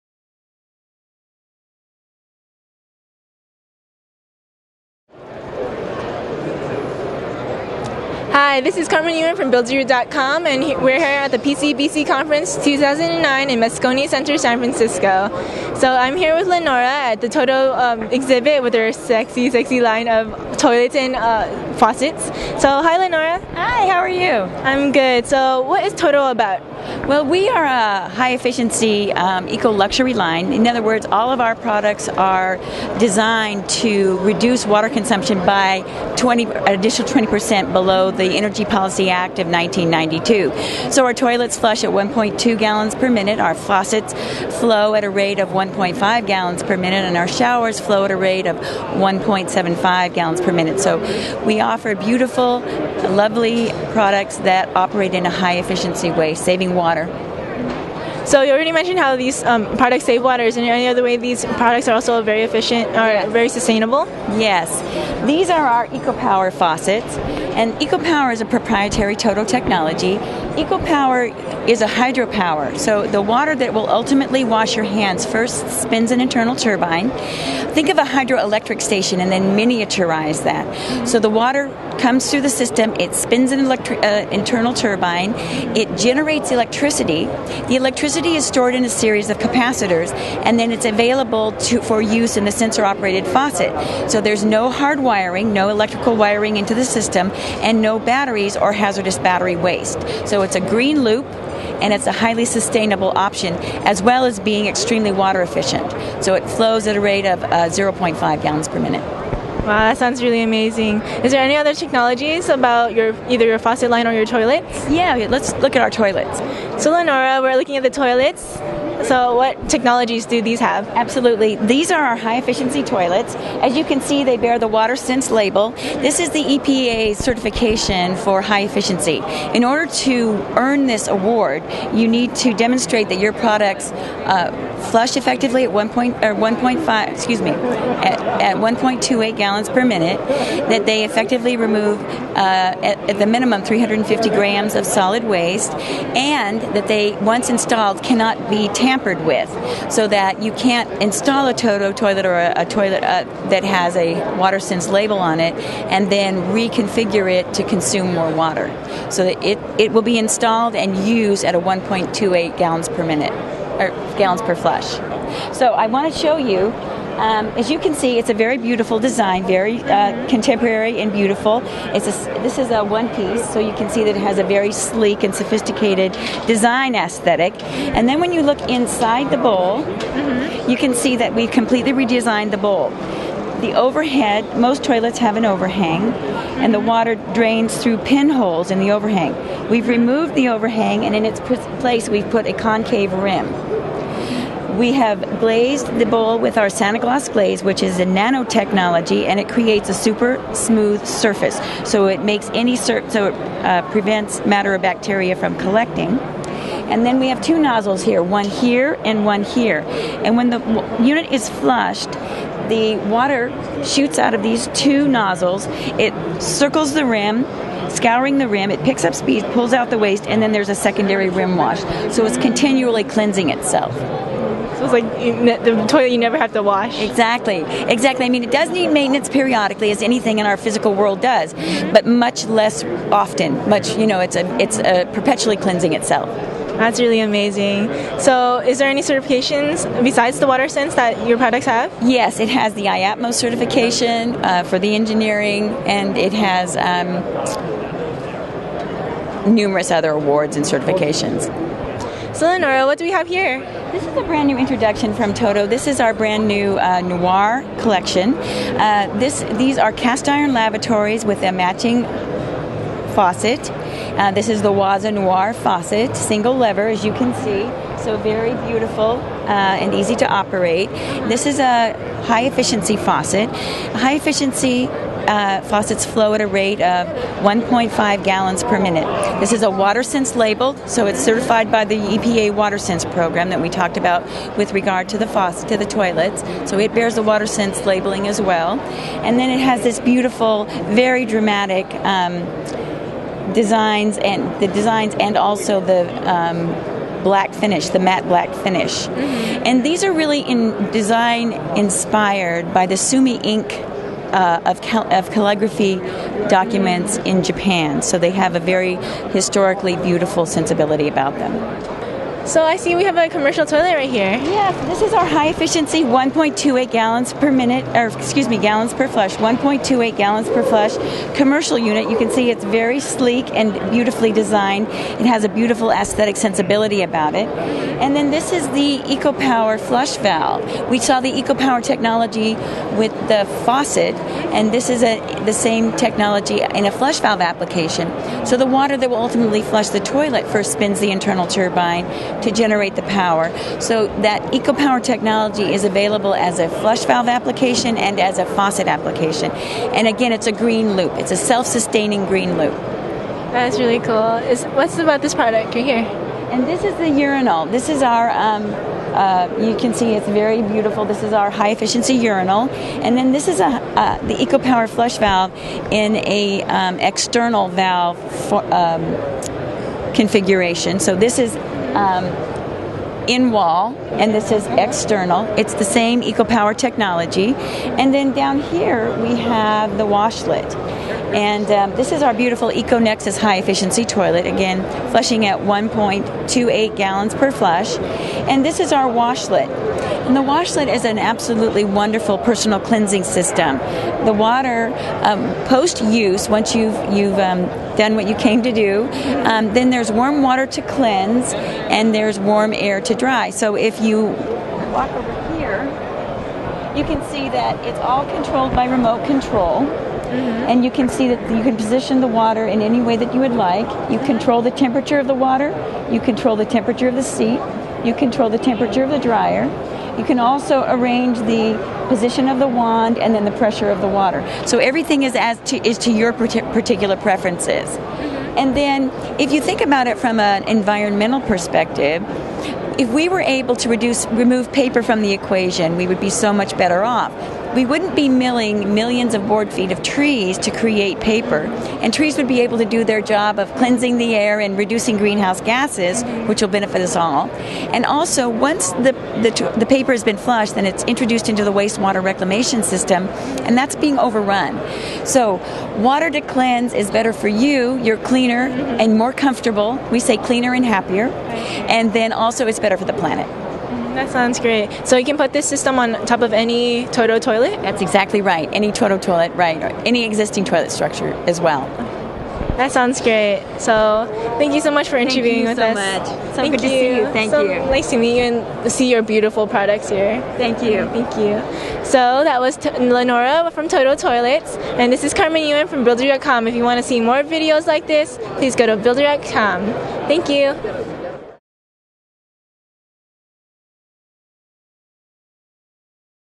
Toto bathroom appliances - buildaroo.com
We love water-saving toilets, so thats why we were at Totos booth with Lenora. Their toilets have EPAs watersense label, and they flush at 1.2 gallons per flush. We are introduced to Totos proprietary ecopower faucet line,  Totos Ultramax toilet, and  Totos Waza Noir Faucet line.  Thank you Lenora for your time; your articulation is superb just like the products you were showcasing.